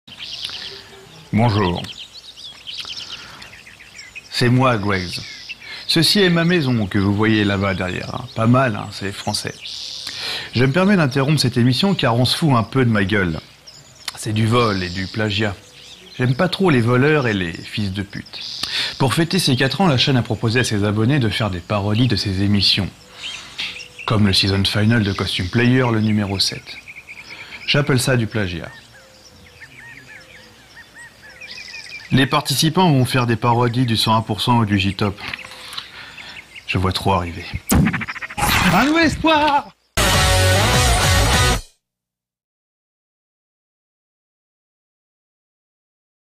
Costume Player pastille pour Les 4 ans de Nolife
J'avais refait un banguplay pour annoncer l'anniversaire de la chaîne;